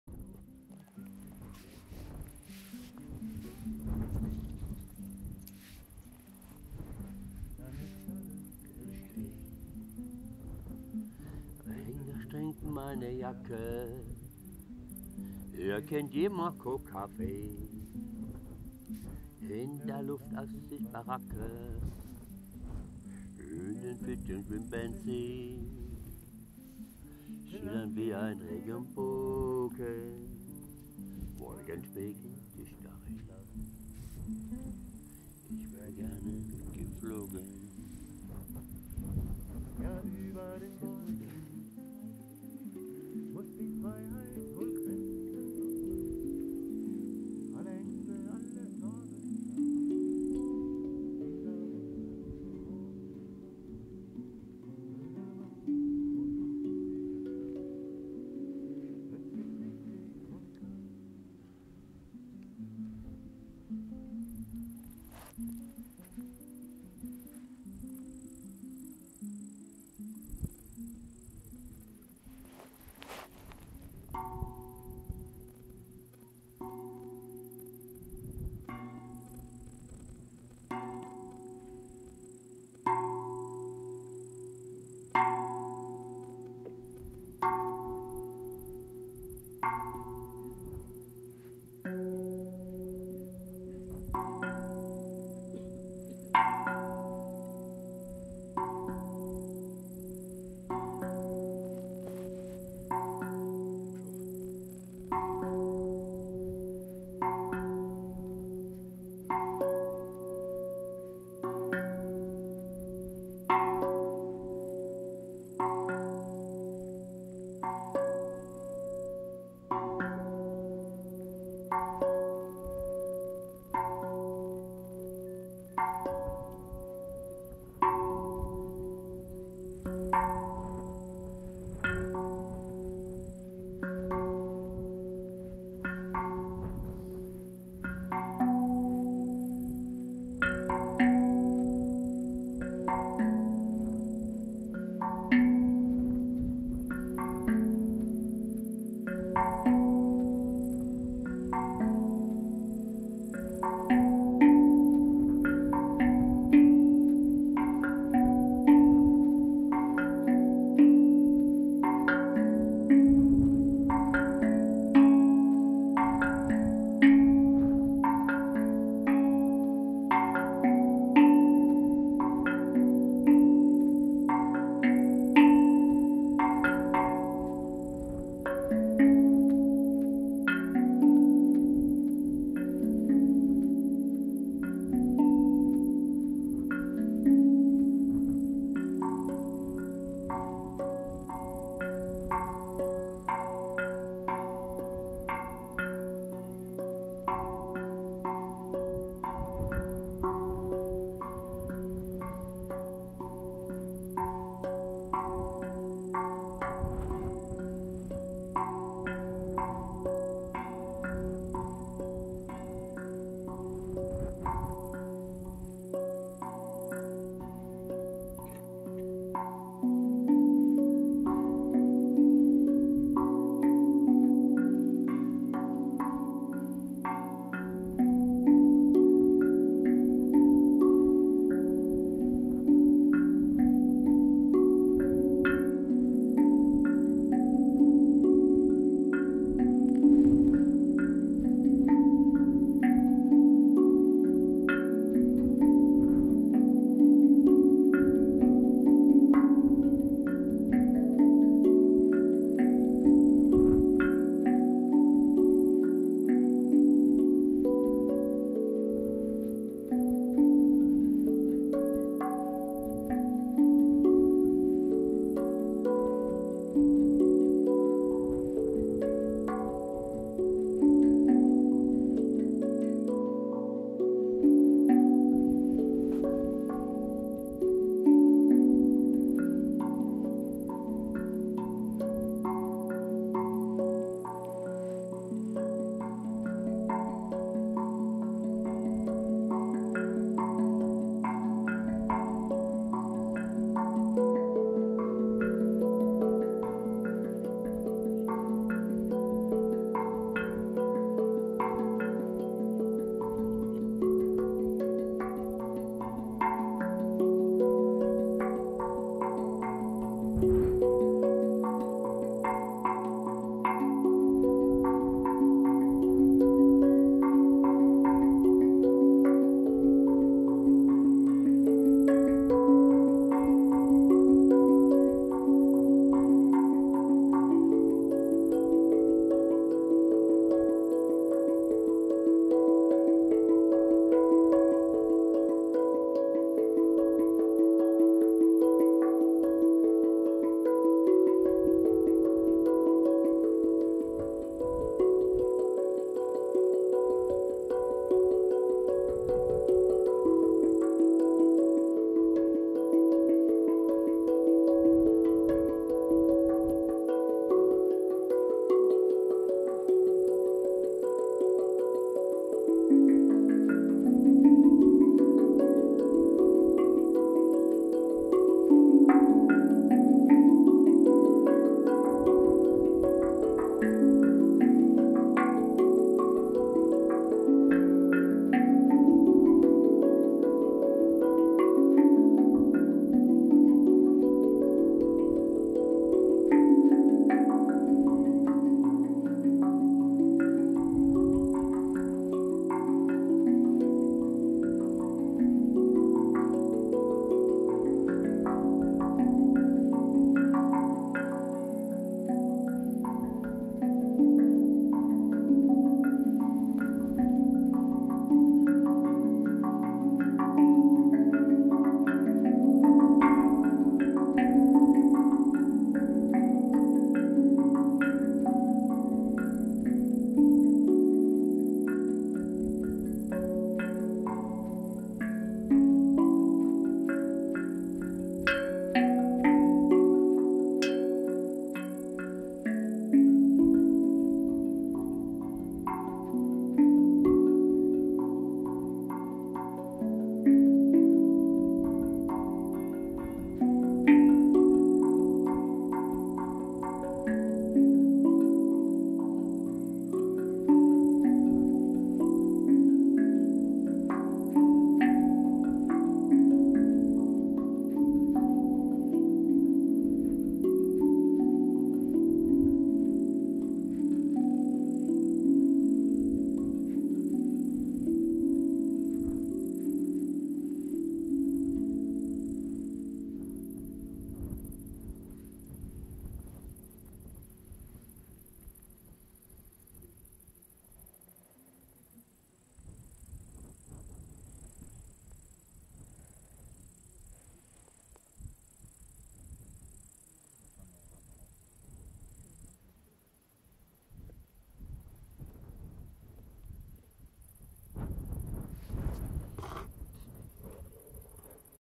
Liquid light Akustik Perseus Polaris auf dem hoher Dörnberg
Wir waren zusammen auf dem hoher Dörnberg mit Löwenherz Andy Gisela